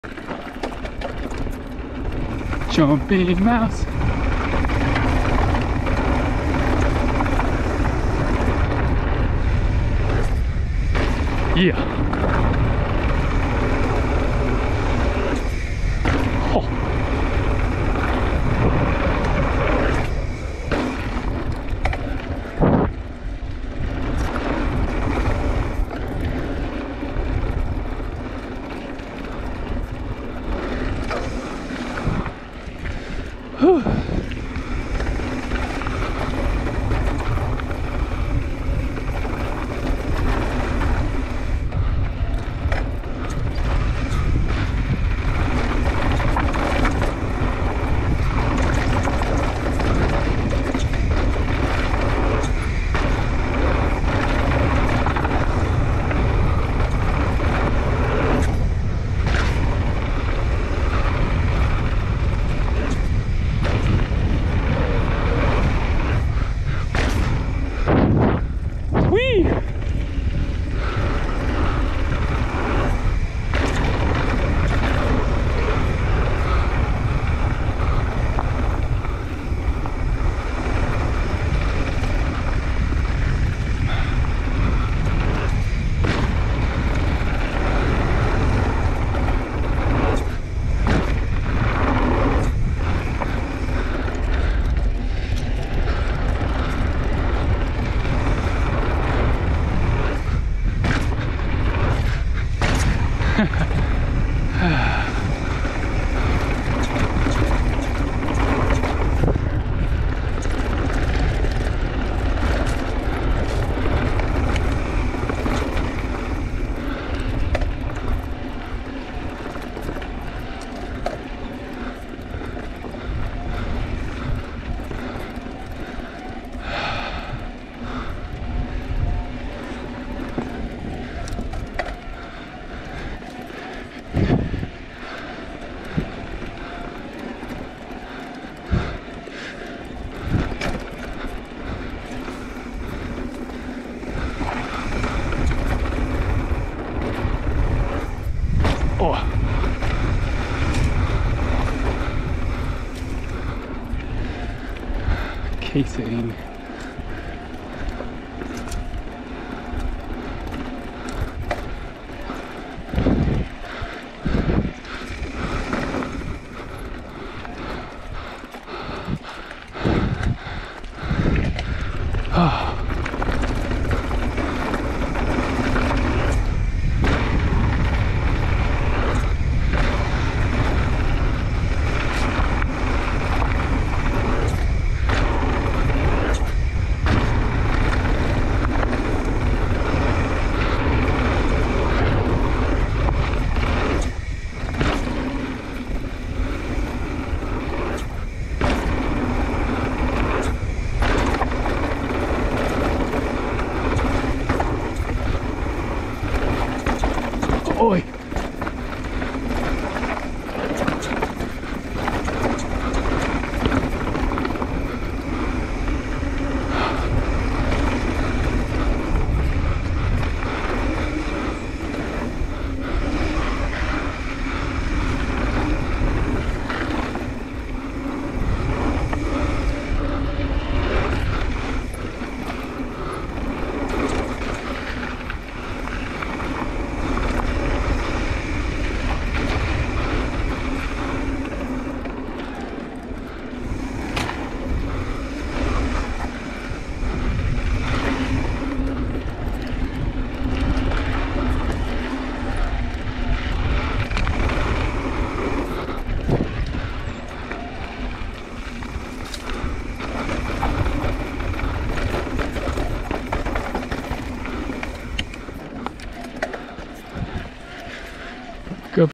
Jumping Mouse // Greer Ranch // 11-15-20 // Turbo Levo SL
A southern California fall classic.  Just lots of fun jumps and high speed flow.  Sorry for the upright camera angle!

Bike:
XL  Specialized Turbo Levo SL
Fork: Manitou Mezzer at 170mm
Shock:  EXT Storia V3 w/Cascade Components linkage and 575 lb/in coil
Wheels:  Mercury Carbon X1
Tires Front:  Vittoria Mazza
Tire Rear:  Vittoria Agarro
Cushcore XC
Cascade Components Linkage
Magura MT7

Camera:
GoPro Hero 8
Chin Mount